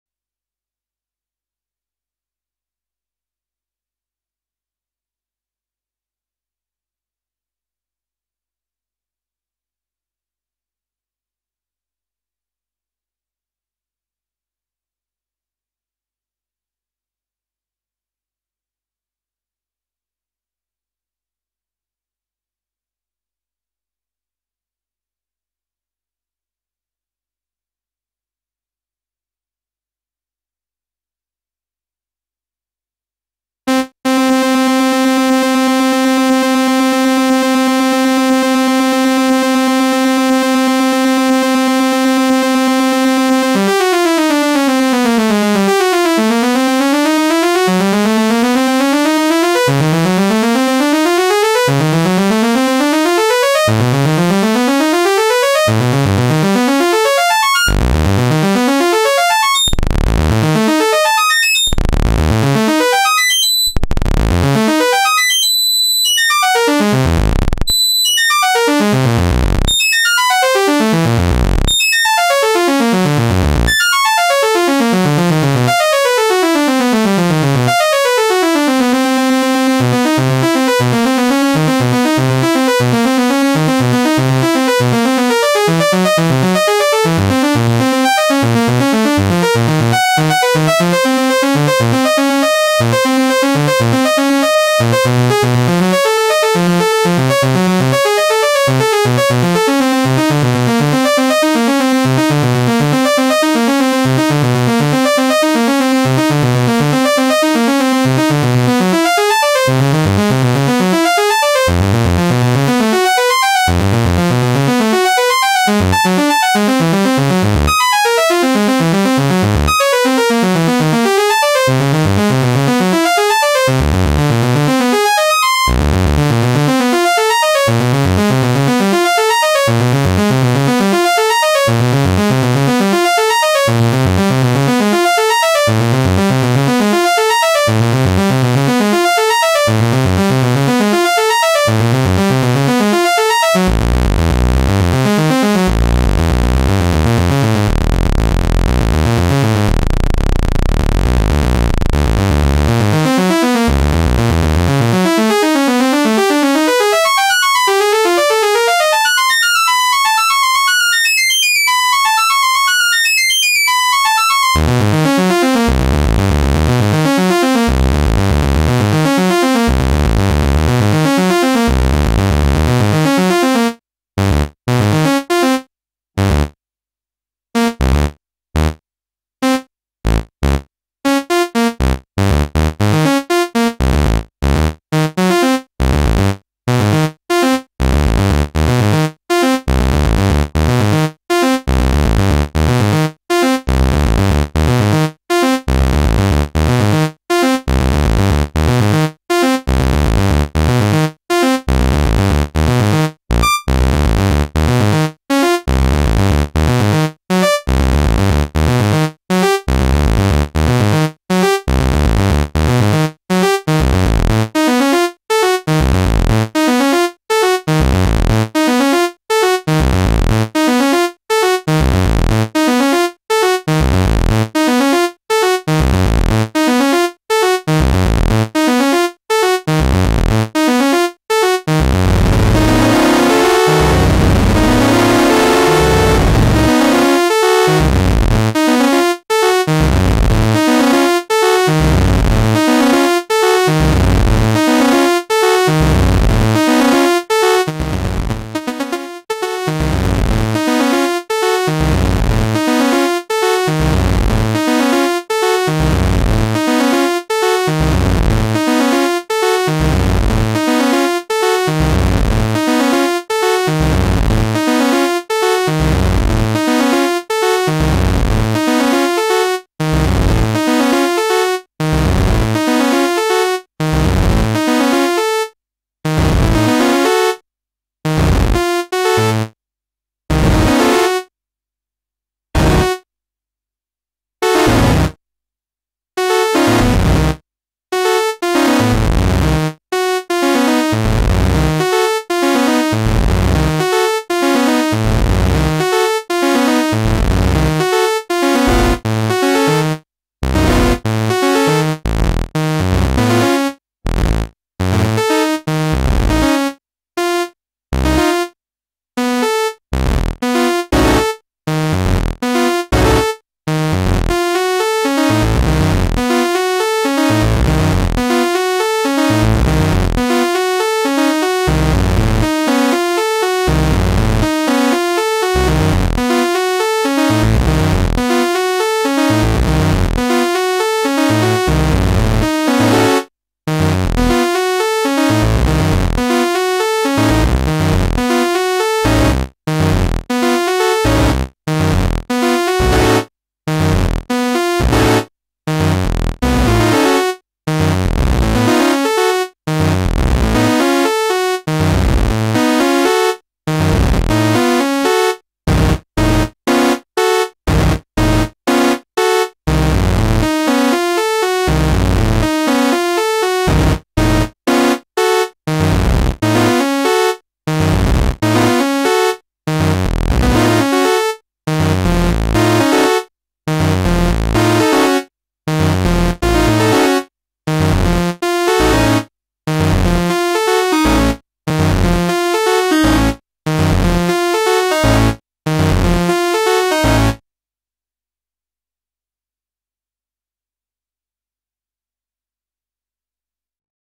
Gotharman's LD3 - Sequencer Templates
Showing a tiny bit of the possibilities with the Little deFormer 3 sequencer templates.